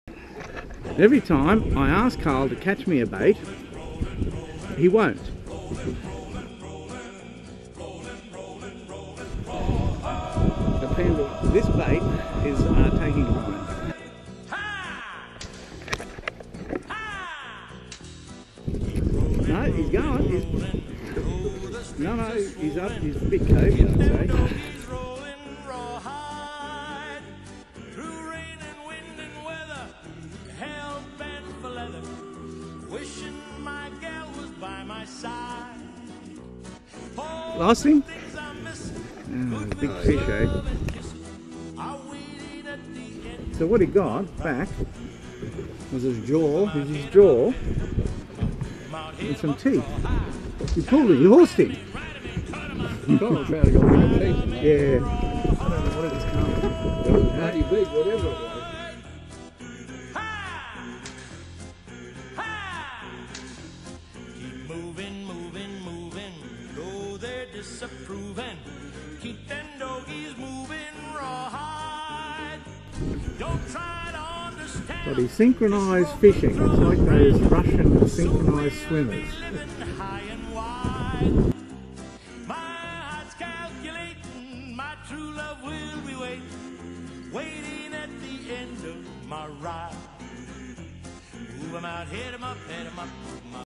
Fishing Sunshine Coast Gneerings reef 1st Jan 2019; Carl catches fish lips
How to catch fish lips off Mooloolaba